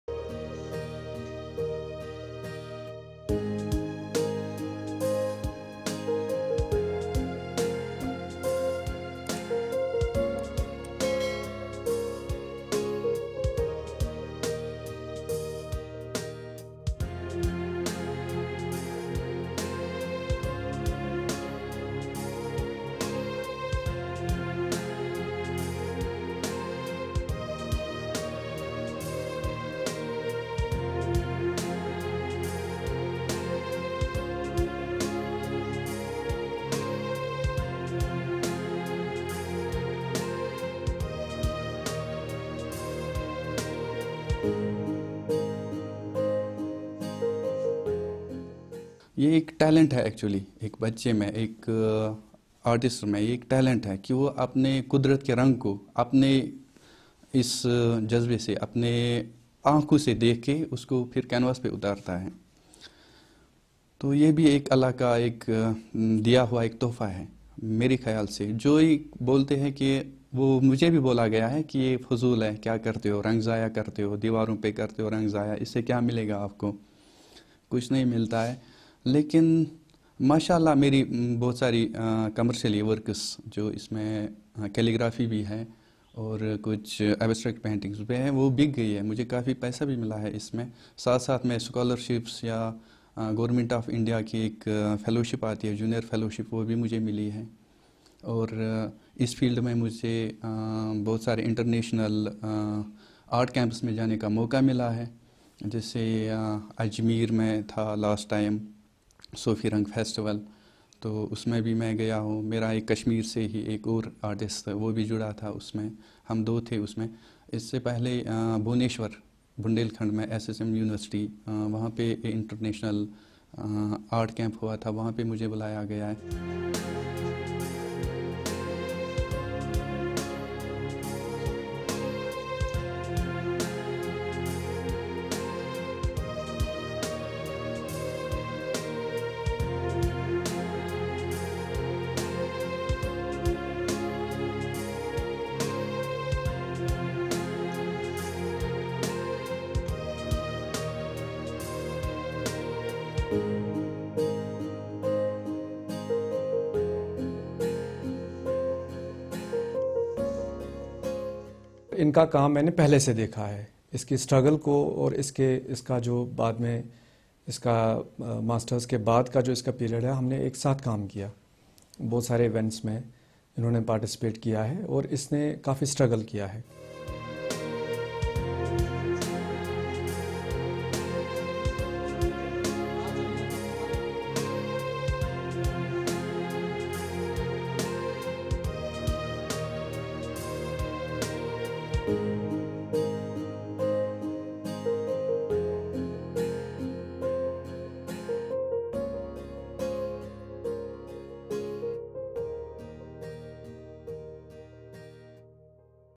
Kashmir’s abstract artist Tasaduq Hussain gets famous at International level
Rising from a small village Magam in Baramulla district, Tasaduq Hussain has made a name for himself as an abstract art painter at International level. However, this journey was not easy for him despite possessing natural talent. Tasaduq Hussain had an inclination towards drawings and paintings since he was very small. He used to take colours of his father to make paintings. His father works as a tailor. However, it was not easy for him to pursue his passion. When he passed out his 12th examination, he wanted to join a fine arts college in Srinagar but almost everyone including friends and relatives suggested otherwise. Nevertheless, he went ahead with his passion and joined the Institute of Music and Fine Arts in Srinagar. Further, he did his post-graduation from Jamia Millia Islamia University of Delhi. Today he has acquired a name for himself. Despite hailing from a poor family, his exhibitions are now well anticipated in both India and abroad. There are many International organisations that call him for an exhibition. People visiting his exhibition enthusiastically buy his paintings. Tasaduq Hussain now wants youth to chase their dreams.